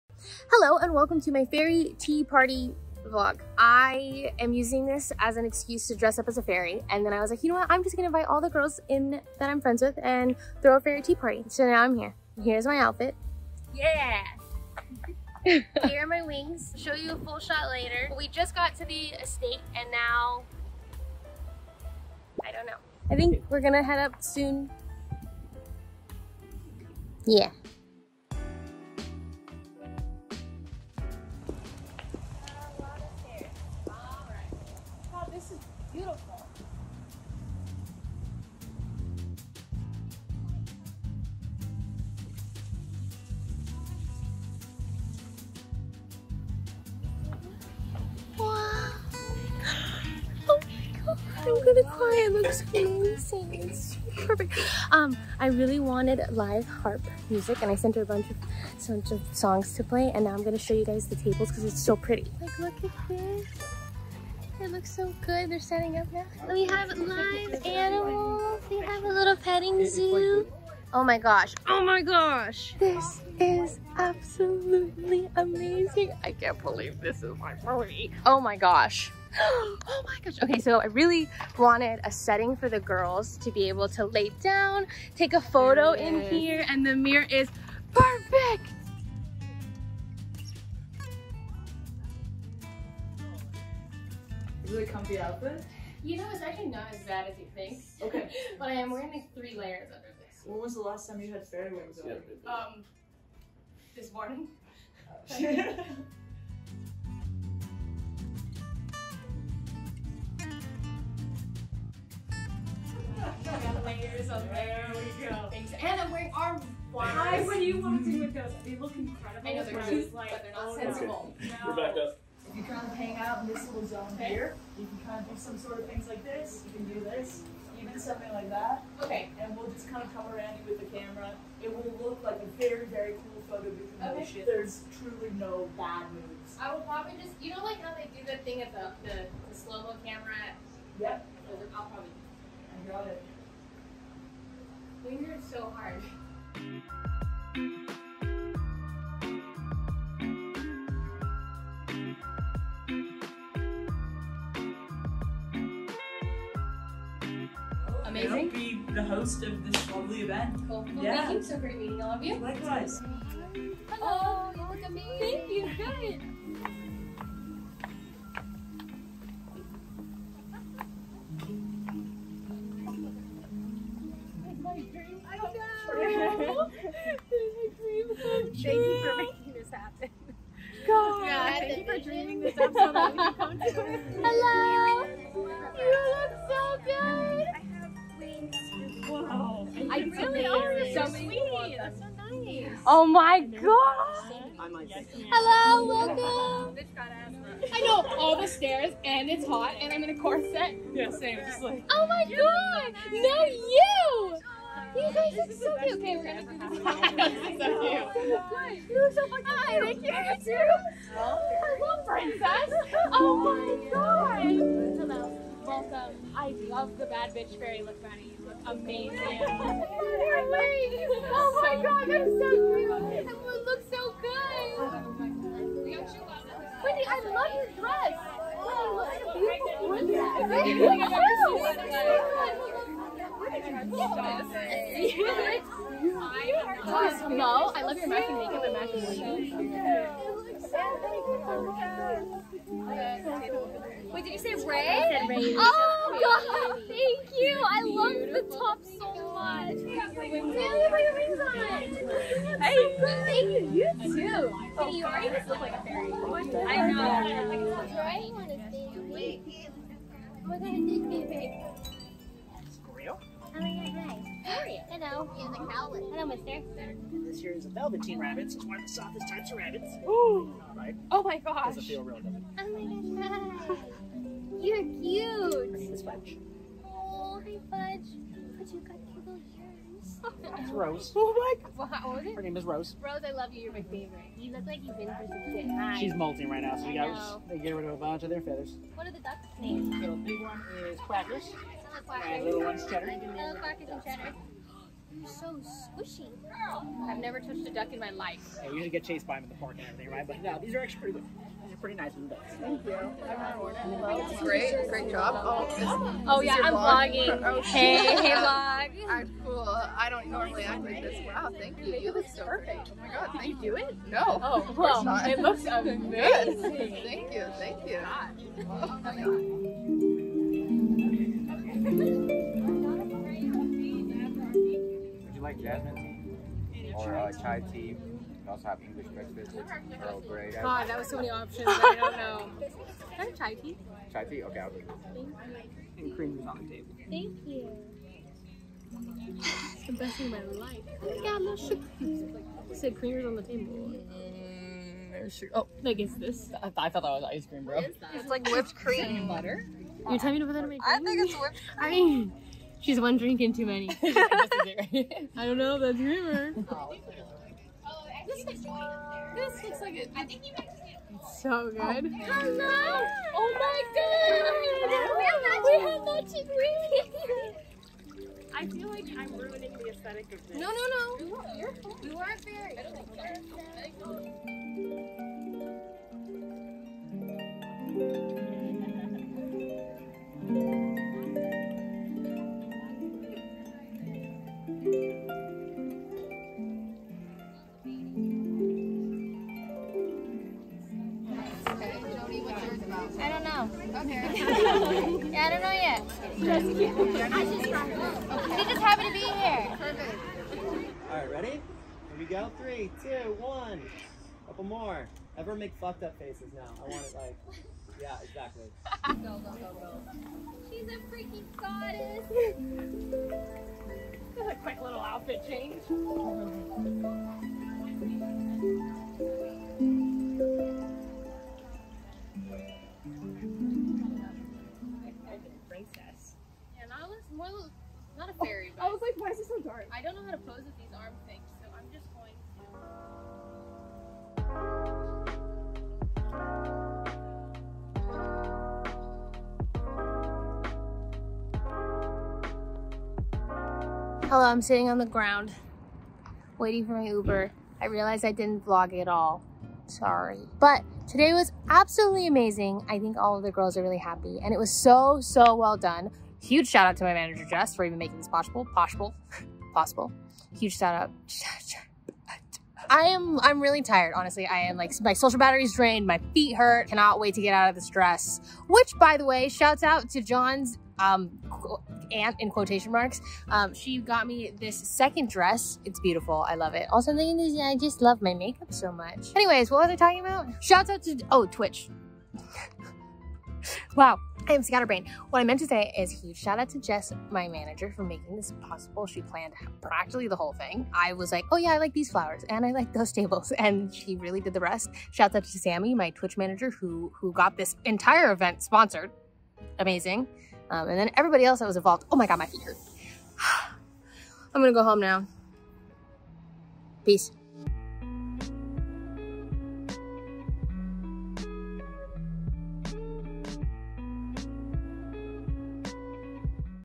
i hosted the fairy tea party of my dreams
a little behind the scenes of the fairy tea party i hosted :)